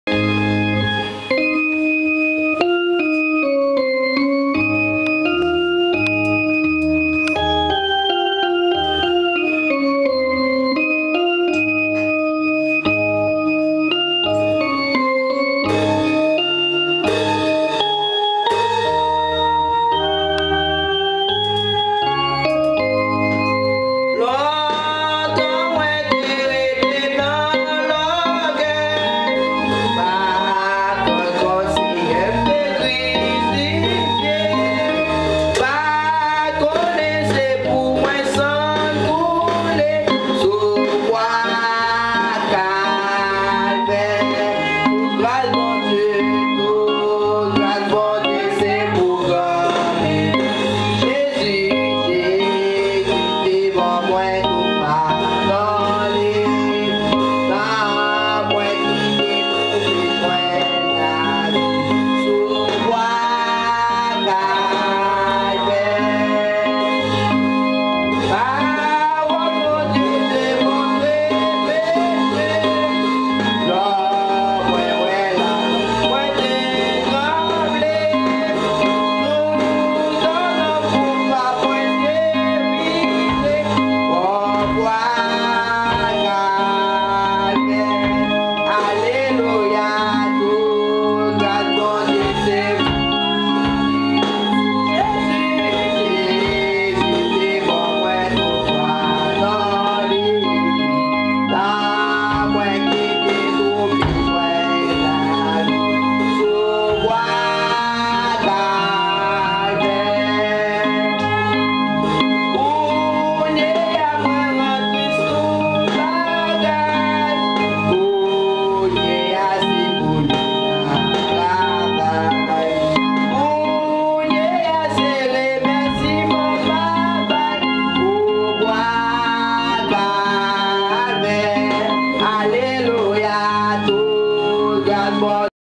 4 yrs old Drummer Jonathan Berard Adoration et Louange Soeur Pierre Berard
Premiere Eglise De Jesus Christ Haitienne
446 south Plank Rd